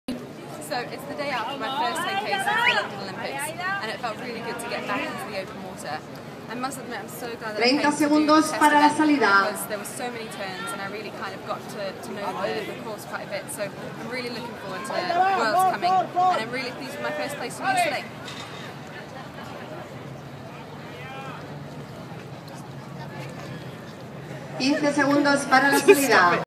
My 10k round up!
A short round up of my 10k from The Barcelona Test Event June 2013

Barcelona Harbour